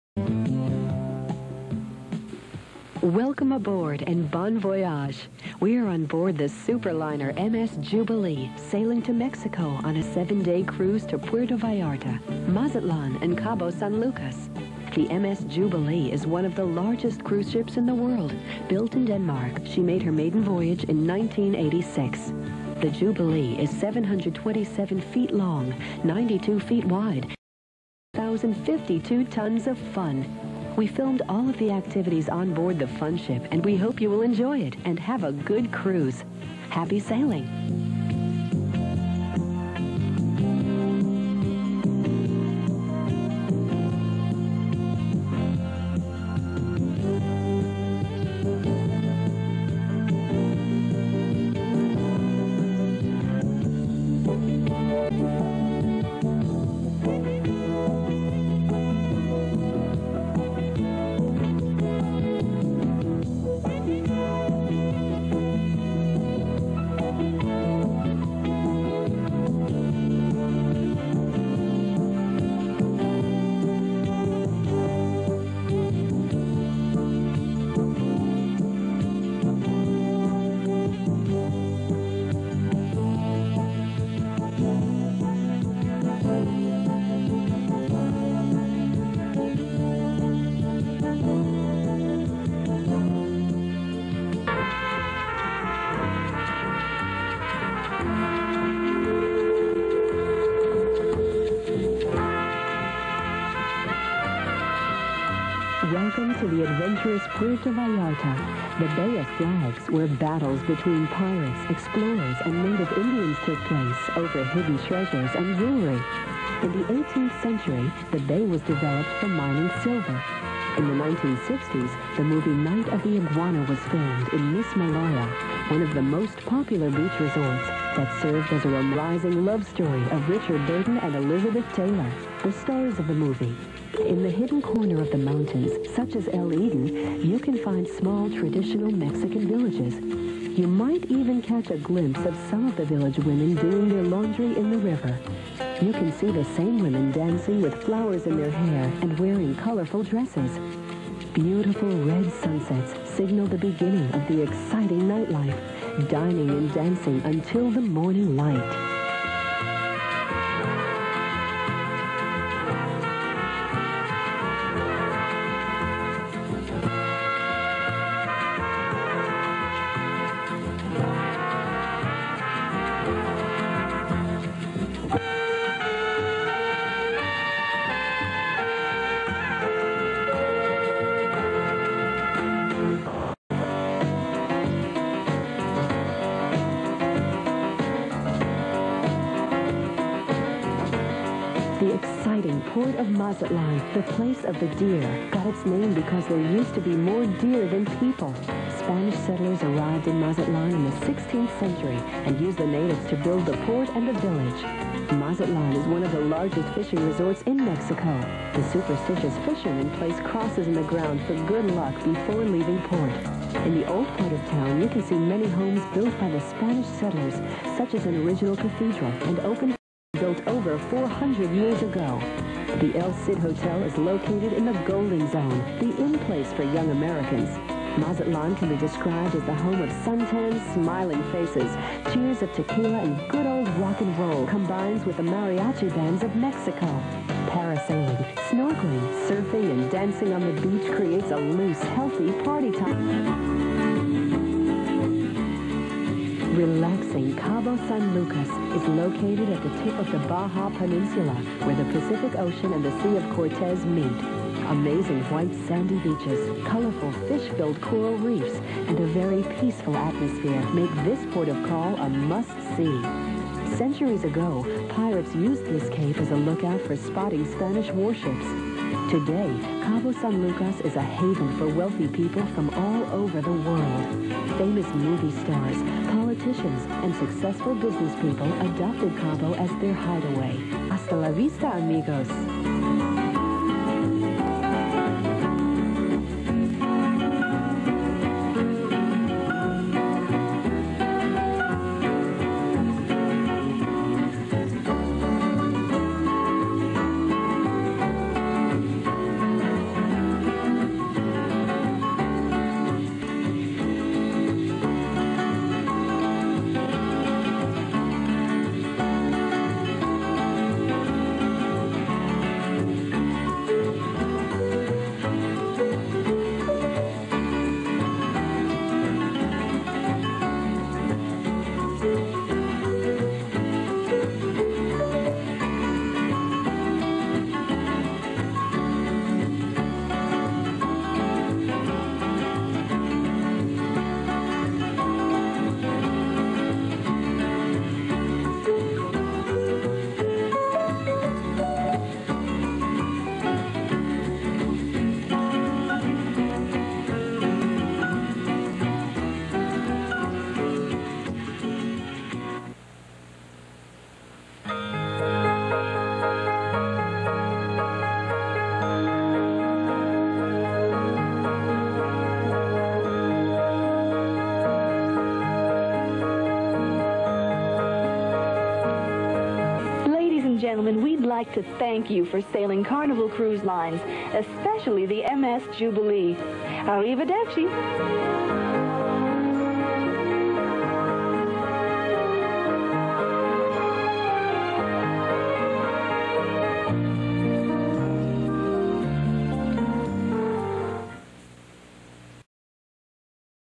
Dr. Patrick Treacy 'Diary of Ship's Doctor' MS Ecstasy
Dr. Patrick Treacy worked as ship's doctor on board the MS Ecstasy from March 1992 until December 1992. These are some of his memories.